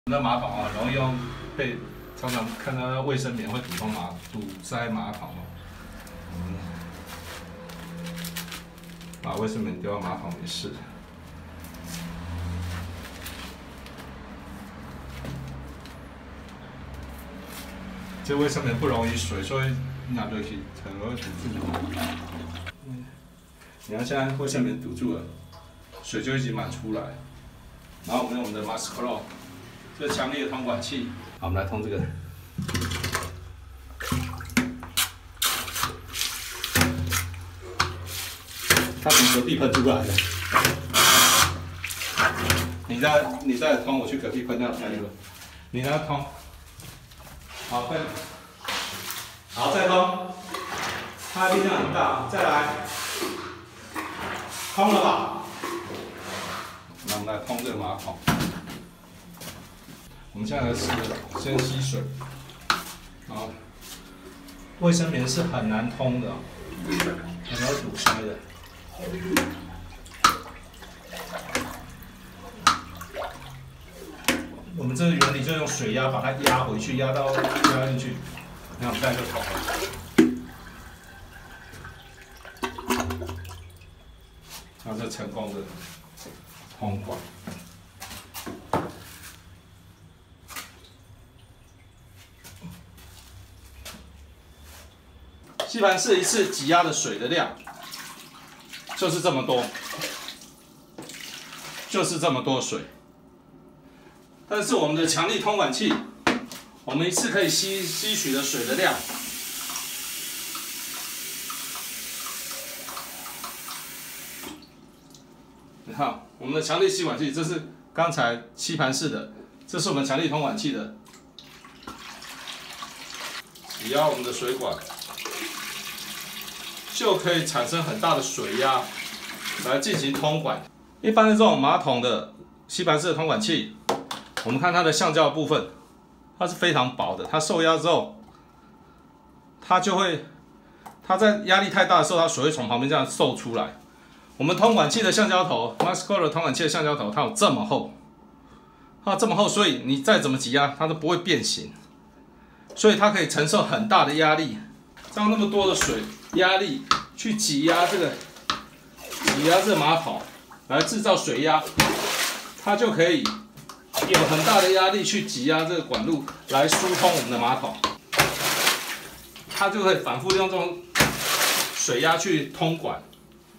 maxclaw強力真空水柱通馬桶器,通管器、馬桶疏通器衛生棉堵塞一樣通
購買連結

超堅固本體、高密合度活塞、強化橡膠真空墊
可以承受較大的推拉壓力=10倍吸盤式通管器
將水吸進圓柱本體，快速推動活塞
一口氣將水往馬桶擠壓，形成水炮通管
反覆吸水、水柱沖擊
活塞吸水時的壓力可以將異物往外吸
鬆動堵塞處的異物，高壓水柱將堵塞處疏通

原理和吸盤式通管器相同，但十倍水量壓力